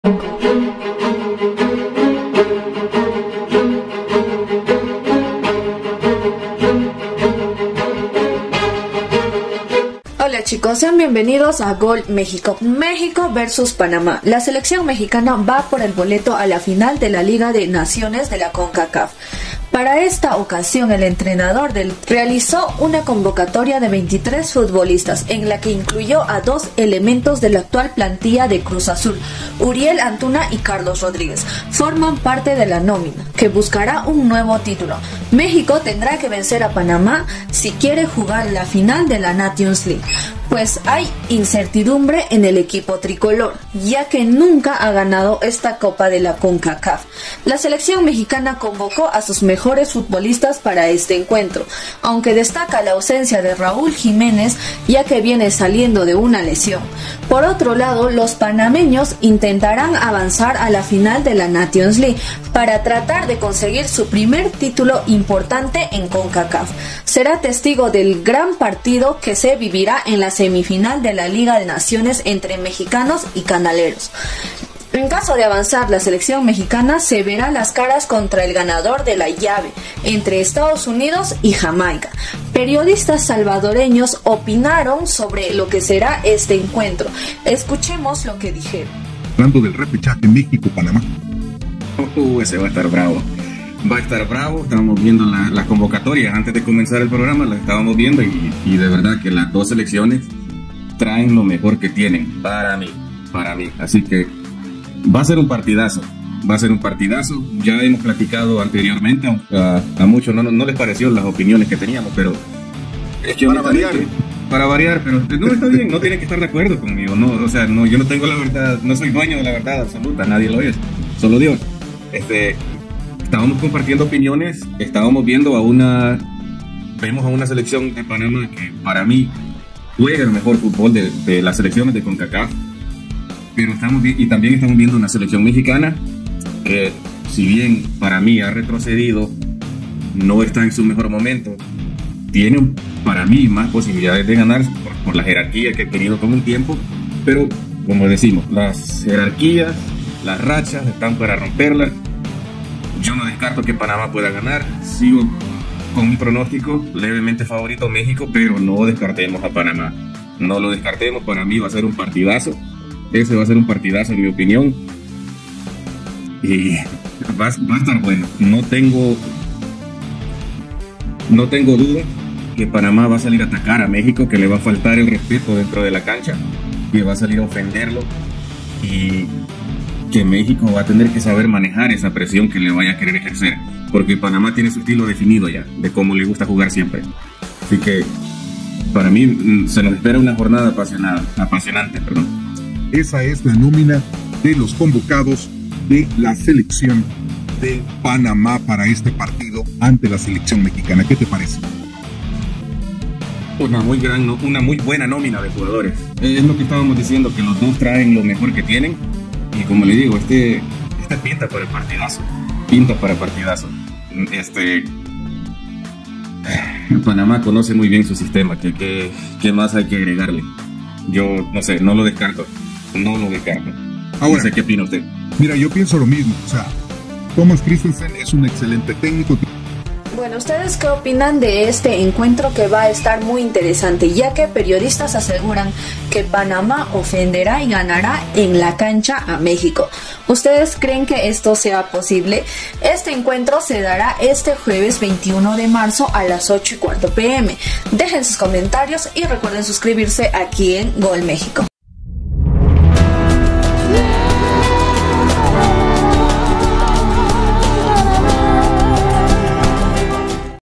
PRENSA SALVADOREÑA DA COMO FAVORITO A MEXICO !! MEXICO SALDRA CAMPEON
PANAMA VS MESXICO SEMIFINAL